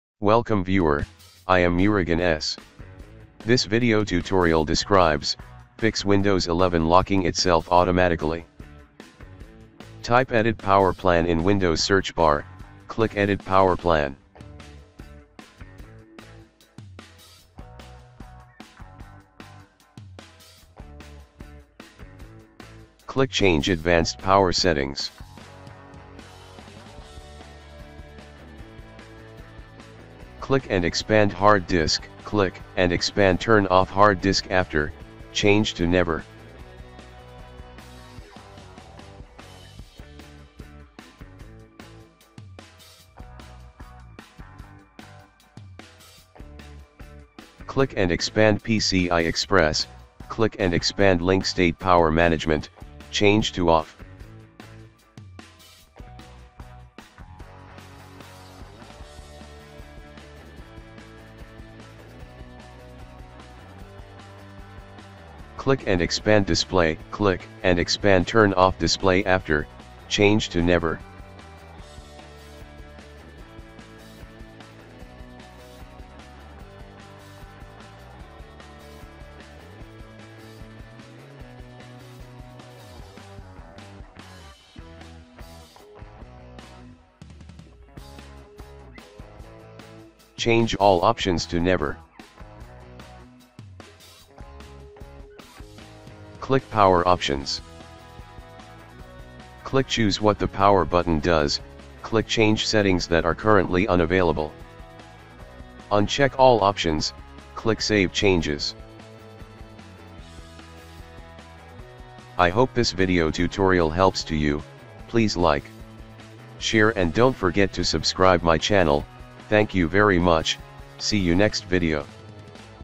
Fix Windows 11 Locking Itself Automatically | Solve Laptop Keeps Locking Randomly In Windows 11/10
This Tutorial helps to Fix Windows 11 Locking Itself Automatically | Solve Laptop Keeps Locking Randomly In Windows 11/10

00:00 Intro
00:09 Edit Power Plan
00:23 Change Advanced Power Settings
01:37 Change Power Options
01:52 Closing